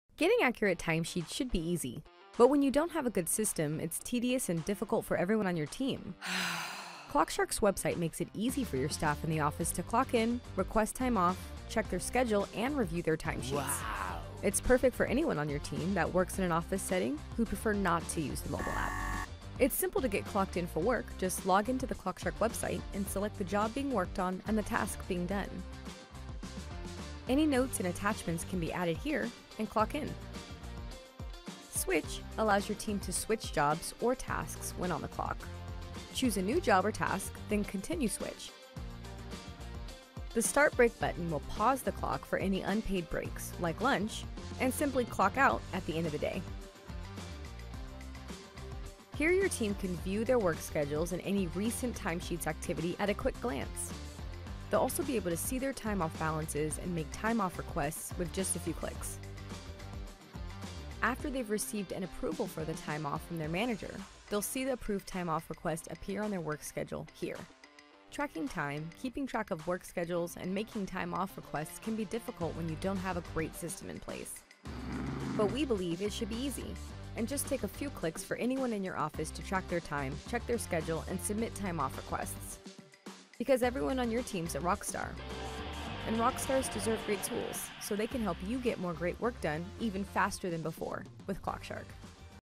An Easy Clock in Solution for Office-based Teams〡Tutorial〡ClockShark〡
In 2022, many companies find themselves with hybrid teams. Some working on various job sites, some from home, and some from the good 'ol office.

Who says there theres not a one size fits all time tracking solution?

When you have office based teams, they can use the web punch clock right from the computer they're already working from. The right web time clock makes clocking in and out, and allocating overhead labor to jobs so simple, your office team will thank you.

0:00 The benefits of using the website
0:19 Using the timeclock
0:48 View schedules and time activity
0:54 Managing time off from the web